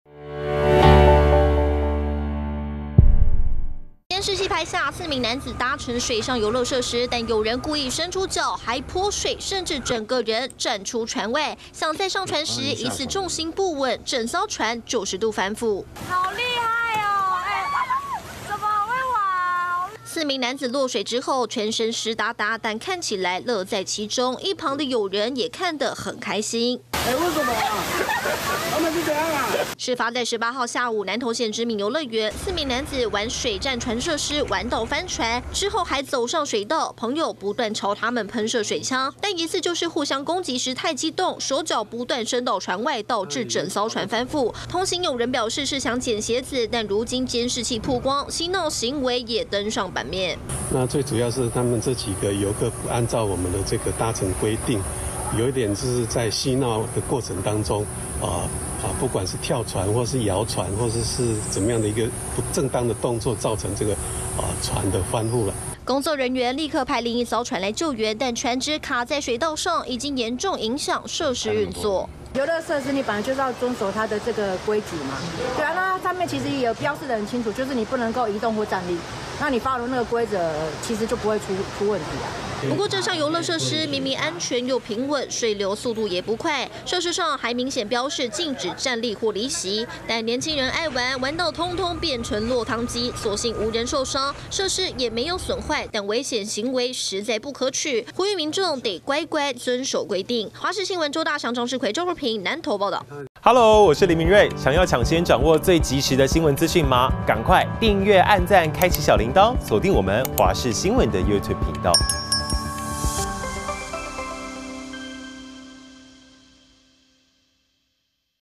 玩水上遊樂設施「跳船翻覆」 4遊客成落湯雞｜華視新聞 20240819
南投縣知名遊樂園，昨(18)日下午發生遊客搭乘水上設施翻船意外，4名遊客落水，園區調閱監視器發現，遊客故意把手腳伸出船外甚至跳船，所幸無人受傷，但也因此影響設施運作，對此園方表示，遊客有嬉鬧行為才會落水，若正常使用相當安全，呼籲民眾還是要遵守規定。